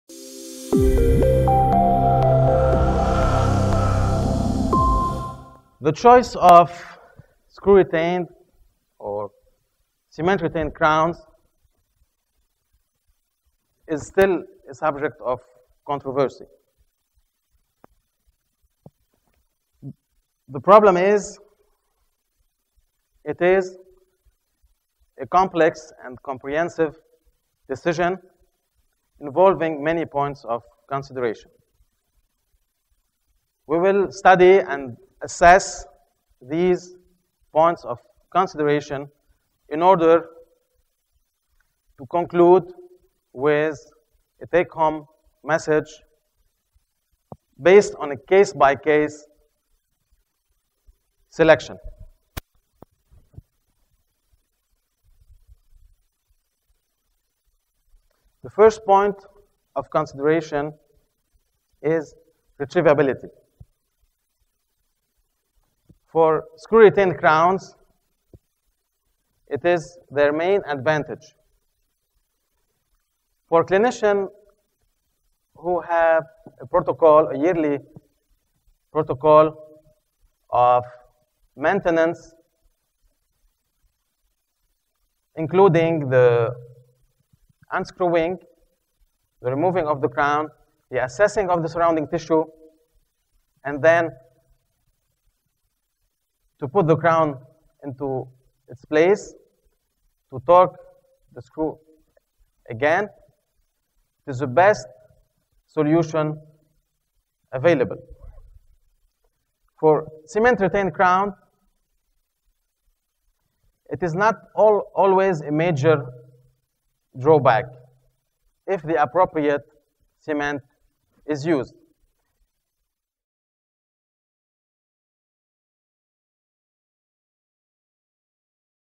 Screw-Retained versus Cemented Prosthesis: Where Do We Stand? | Alain Abi Sleiman
ITI Academy Congress Lecture Preview: Screw-Retained versus Cemented Prosthesis: Where Do We Stand? by Alain Abi Sleiman.